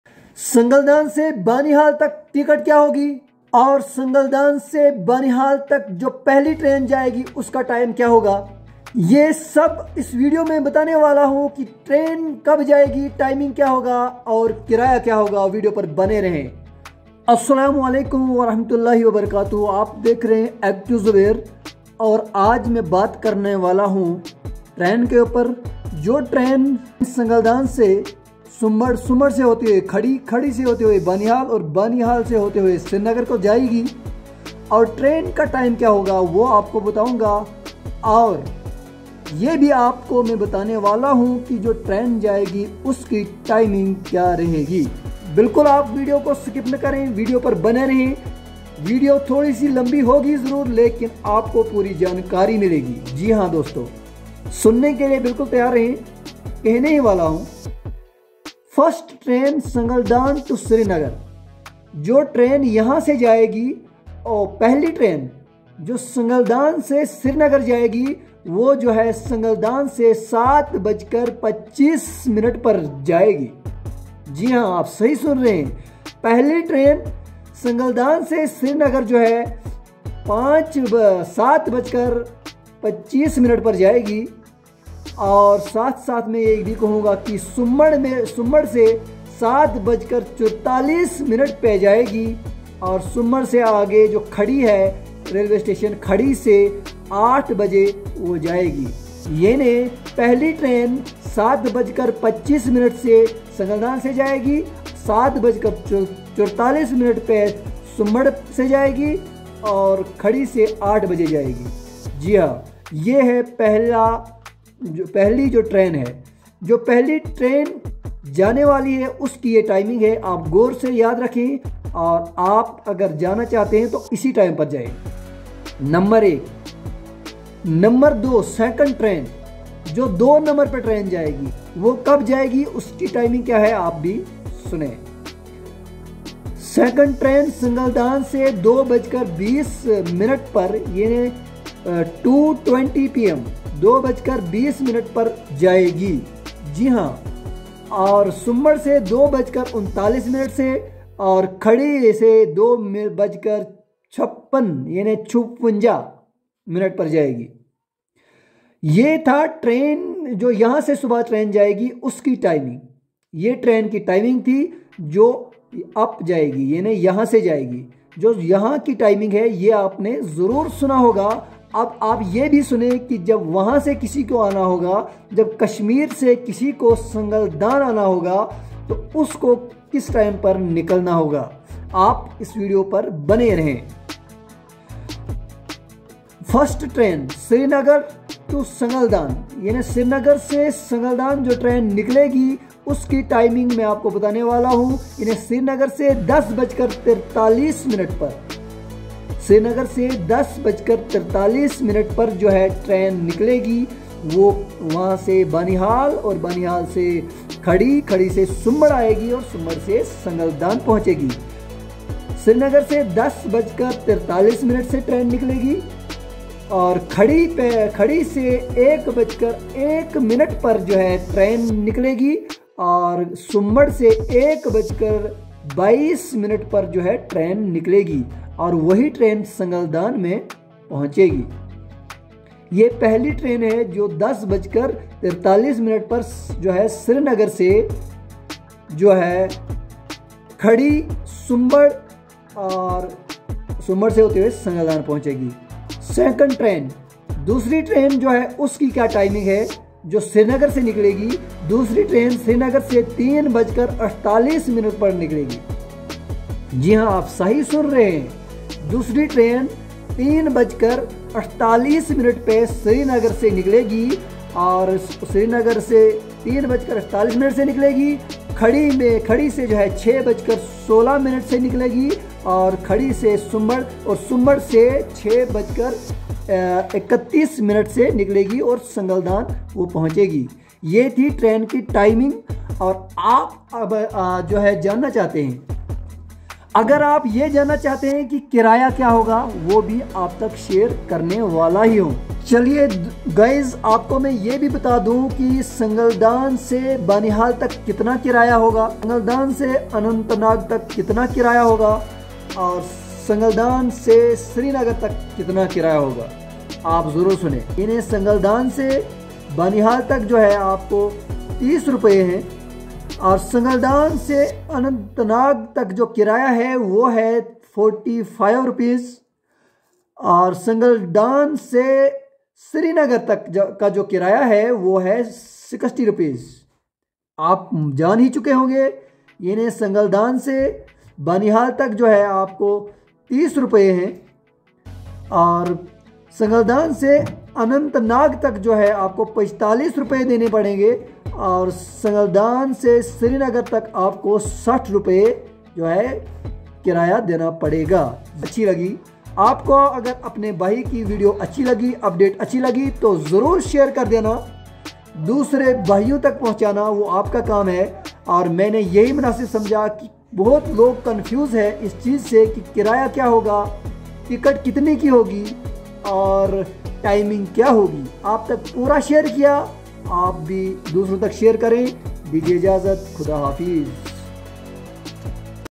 Morning train timing //singaldan to Srinagar//srinagar to Singaldan full update
سنگلدان سے سری نگر تک ٹرین کا کرایہ کیا ہوگا ٹرین کتنے بجے جائے گی